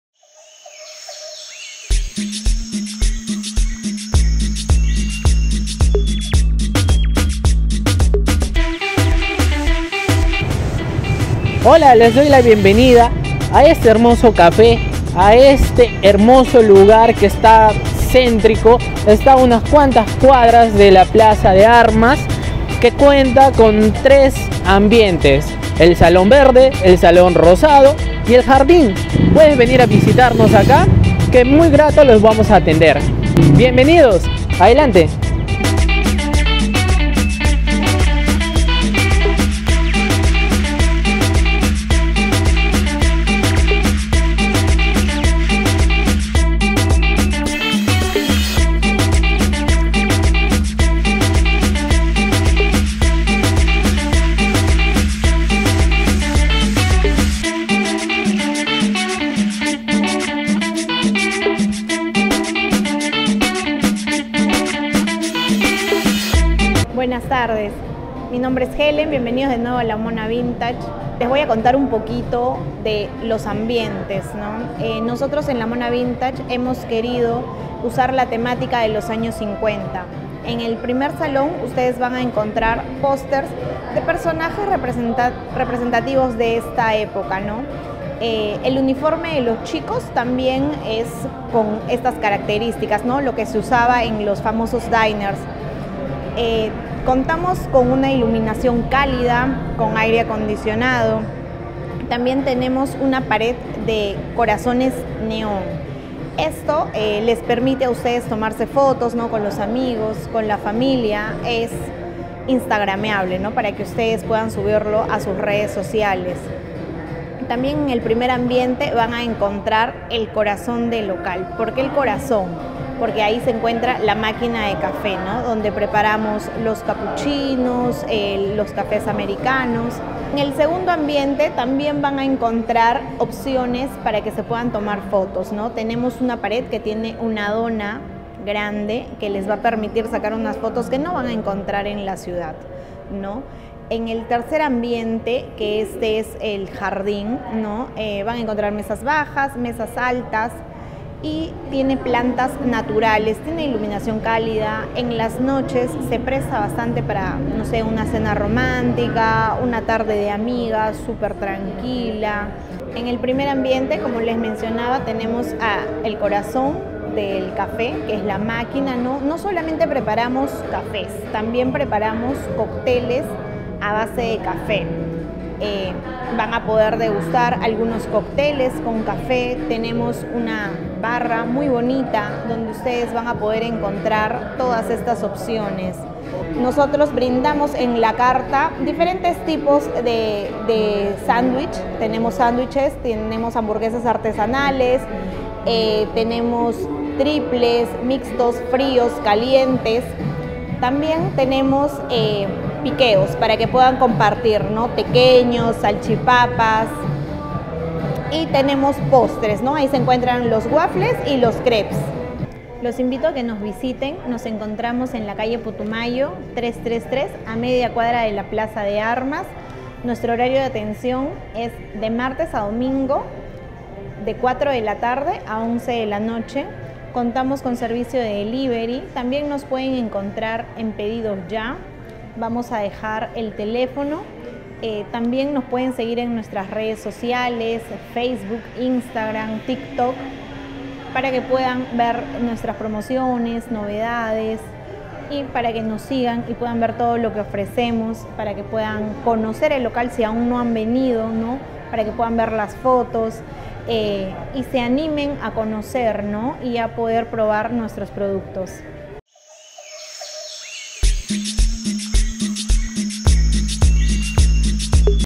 La Mona Vintage - LUCES Y SOMBRAS DE LORETO.
En esta secuencia conoceremos un ambiente amplio, fresco y entretenido para tener una reunión de amigos, celebrar un acontecimiento especial y llevarse una hermosa foto.Nuestra comadrita Helen Figueroa, nos enseña los tres ambientes dónde podrás disfrutar de los ricos:

- Milkshake´s
- Frappés
- Cafés
- Jugos
- Waffles
- Sánwiches
- Salchipapa
- Hamburguesas y más.

Atención de 🕗 Martes a Domingo de: 4:00 pm - 11:00 pm
En📍 Jr. Putumayo #333.